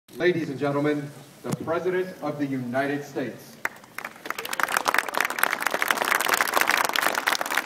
obama-walks-out.MOV
President Obama walks out to deliver remarks before signing the JOBS Act, April 5 2012